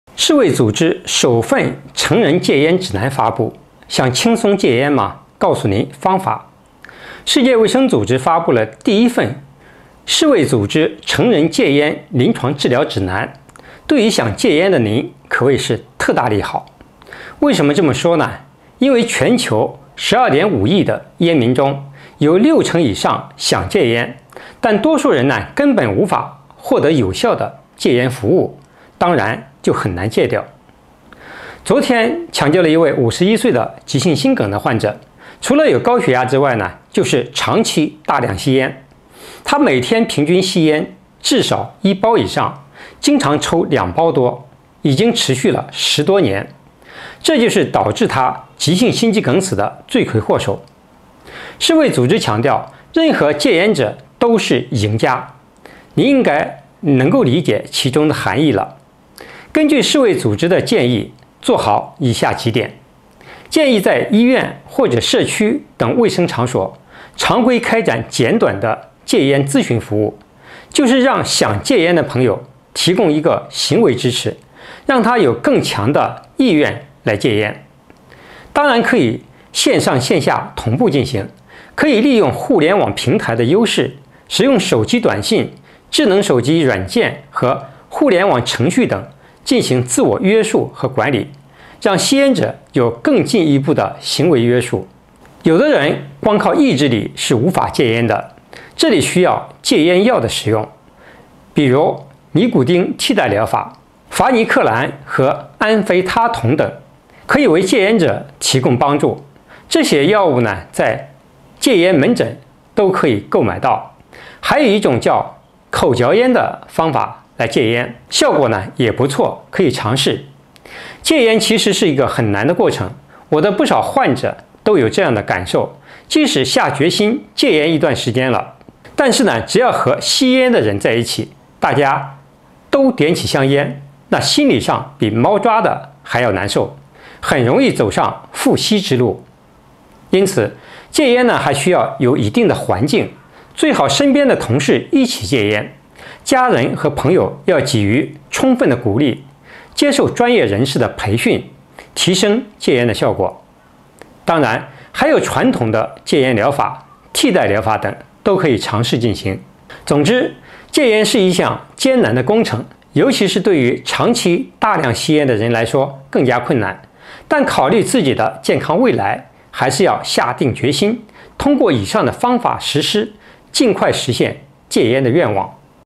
世卫组织首份成人戒烟指南发布，想轻松戒烟吗？告诉您方法
世界卫生组织发布了第一份《世卫组织成人戒烟临床治疗指南》，对于想戒烟的您，可谓是特大利好。